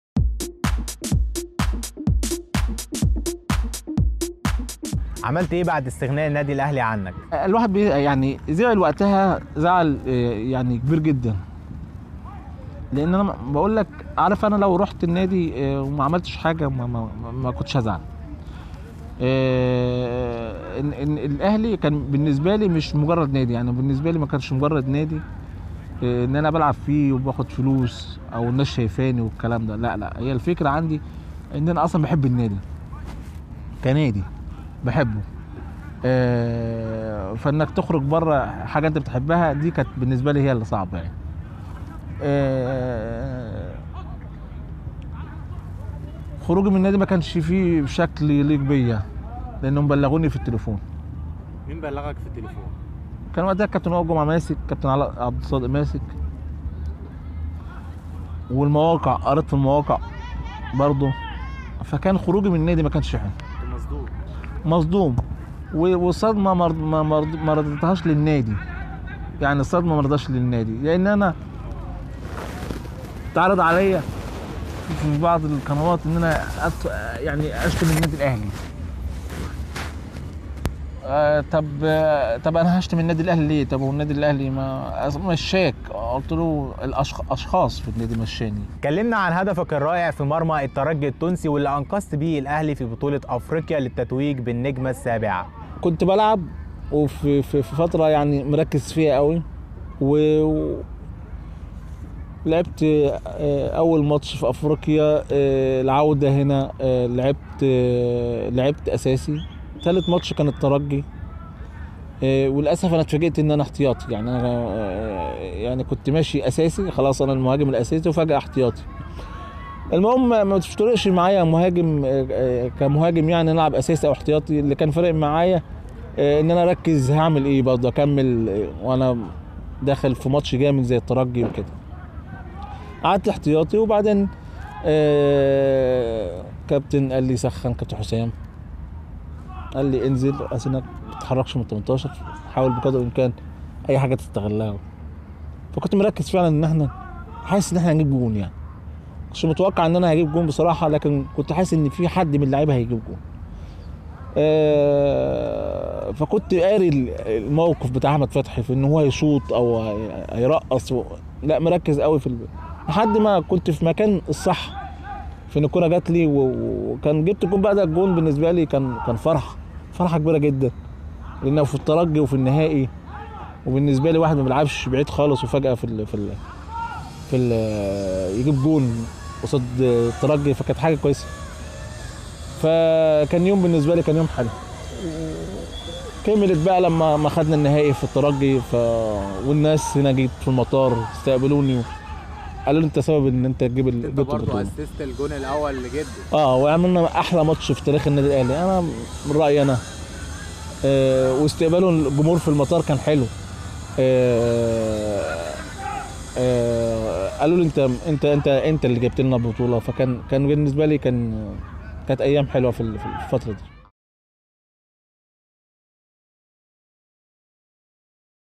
السيد حمدي: اتعرض عليا من بعض القنوات اني أشتم الأهلي.. واستنيت إصابة ٥ مهاجمين في الفريق علشان ألعب
حوار
مصطفى غطاطي
تصوير
أحمد حسين
روان محمود
مونتاج
أحمد عبدالشفيع